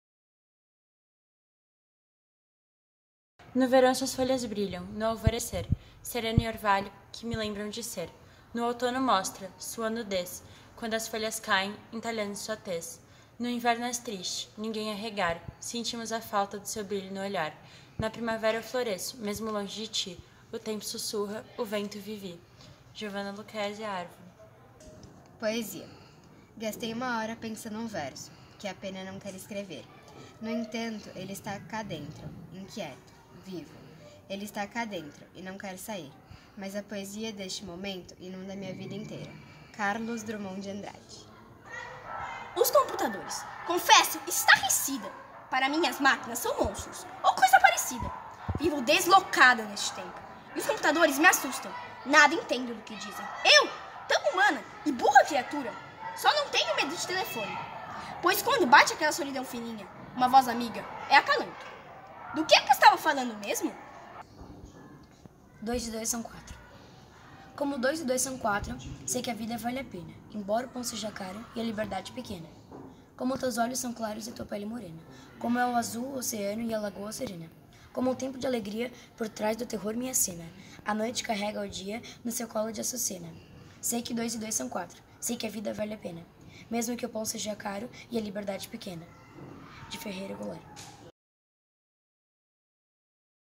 ENCONTROS POÉTICOS 2018 NO F2
Nos dias 25, 26 e 28 de junho, a Escola Viva receberá pais, alunos e funcionários em um espaço reservado para ler uma poesia, para tocar uma música e para trocar experiências literárias. Uma confraternização poética para trazer inspiração para nossas vidas.